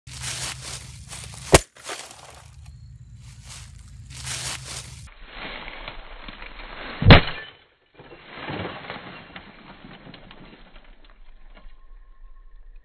Mini CRT TV Smash
I was bored and decided to smash this old tv